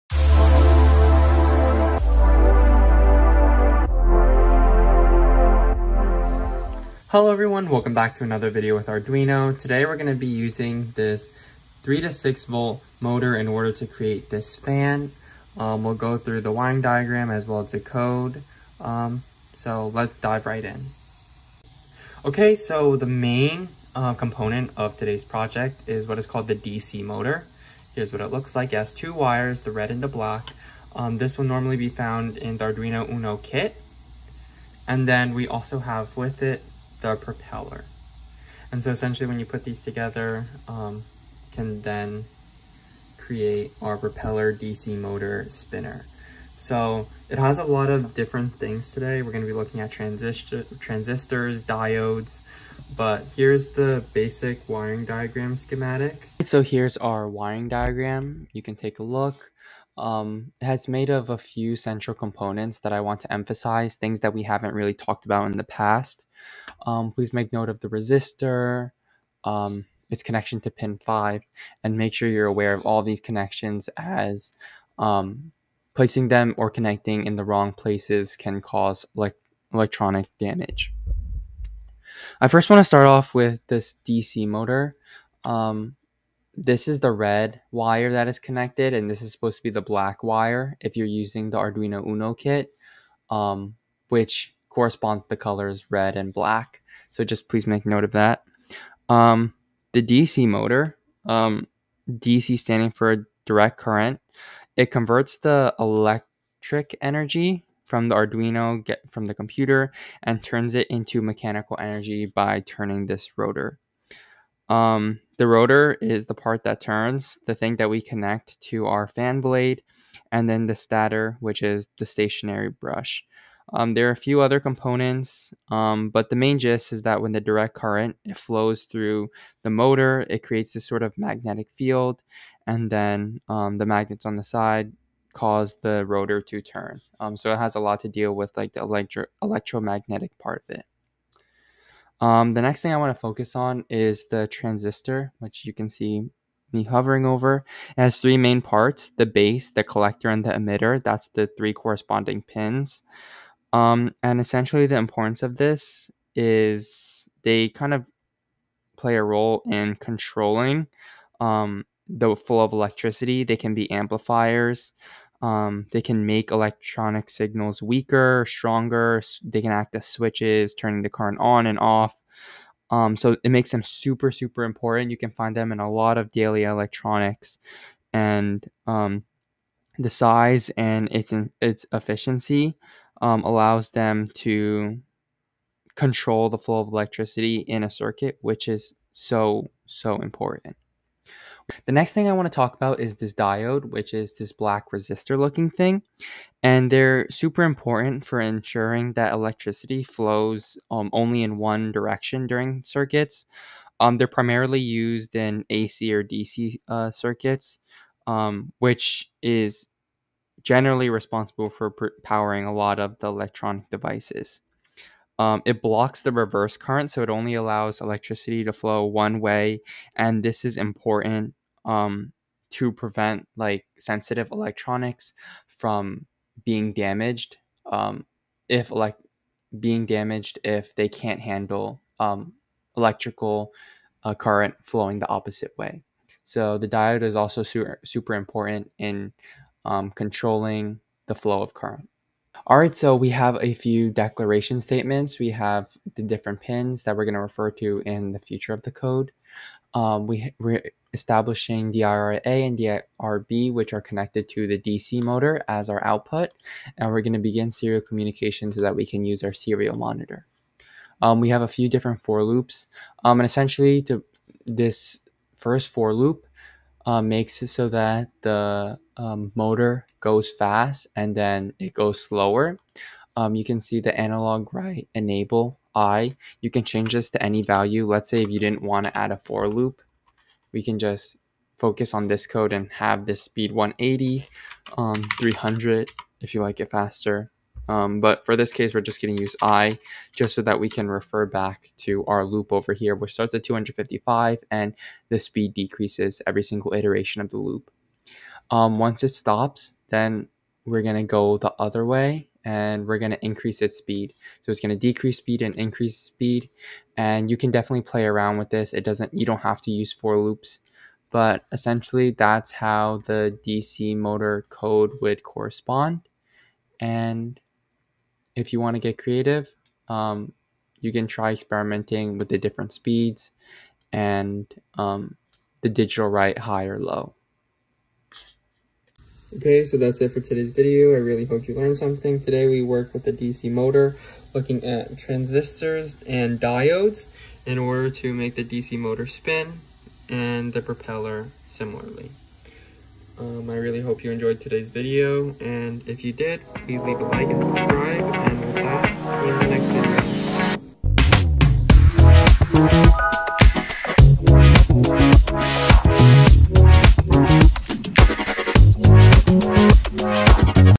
DC Motor Fan - In depth and WORKING Tutorial
In this video, we looked at a project using the DC motor and how we can add add a fan to make a functioning DC motor fan. We walked through the code and logic to show how the DC motor works as well as the other components of the project which are transistors and diodes.